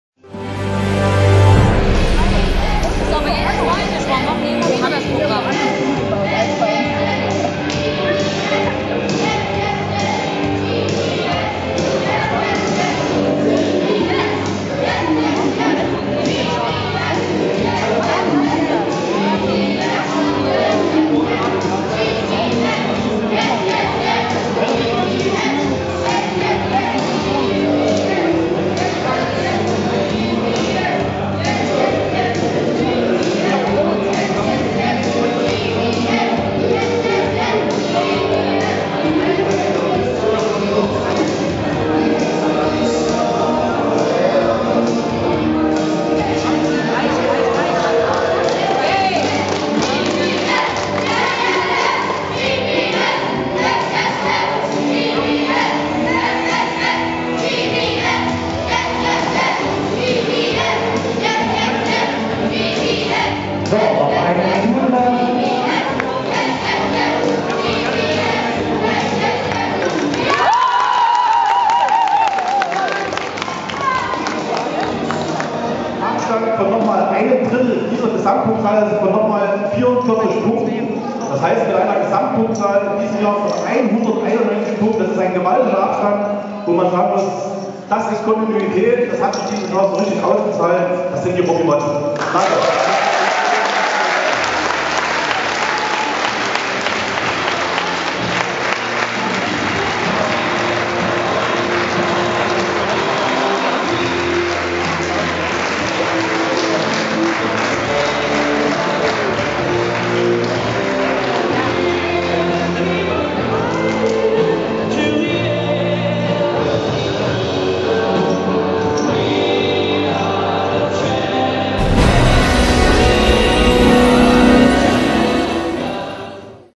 FLL2014-CHAMPION Zwickau: Robymotten
FLL2014-CHAMPION im Regionalwettbewerb Zwickau für die Robymotten des Julius-Motteler-Gymnasiums Crimmitschau